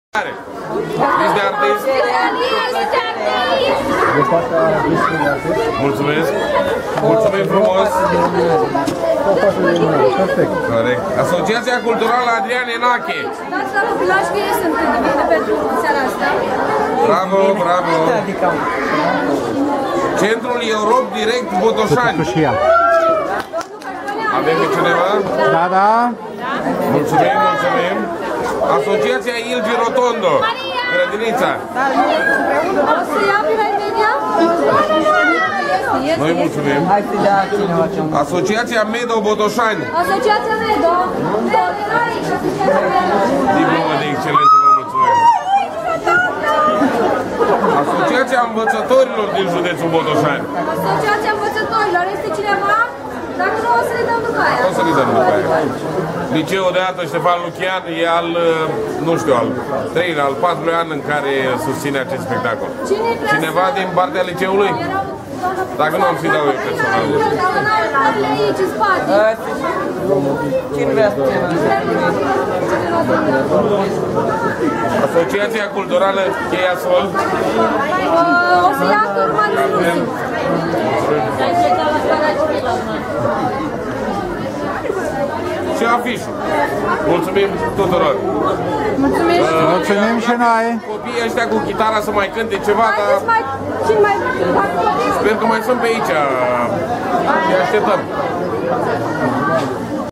Ora pământului 2012 Botoşani: diplome din partea Primăriei
Viceprimarul Cătălin Alexa acordă Diplome din partea Primăriei Botoşani.